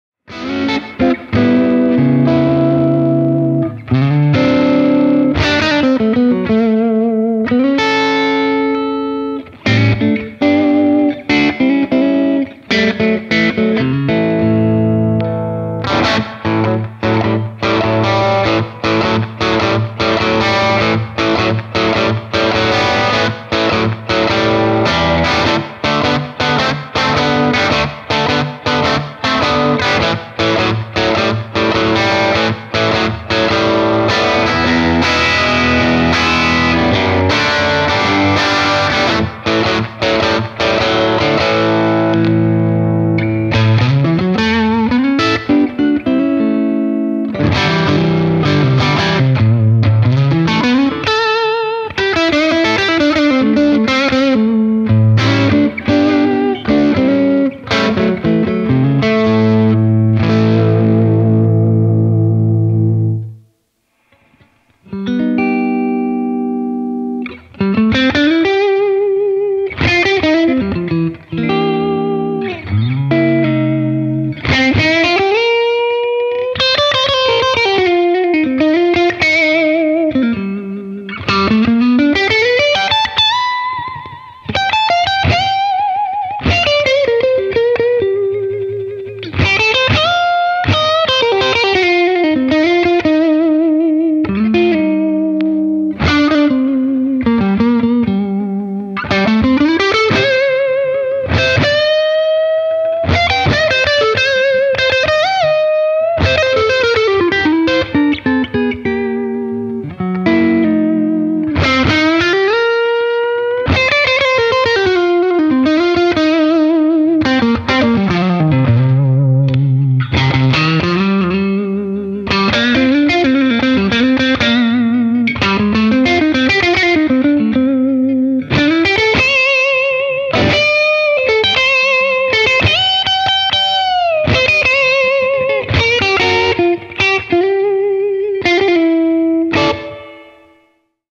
Retrograde Lexington w/Seth Rosenbloom @ The Music Emporium
Boston-based blues wizard Seth Rosenbloom stopped by the shop the other day to take our new Retrograde Lexington #1 solid-body for a socially distanced spin via a Magnatone Varsity Reverb, no pedals.

This guitar has been sold, but you can check out our current and past Retrograde stock here:

Amp: Magnatone Varsity Reverb
Recording Rig: Universal Audio Ox
Pedals: None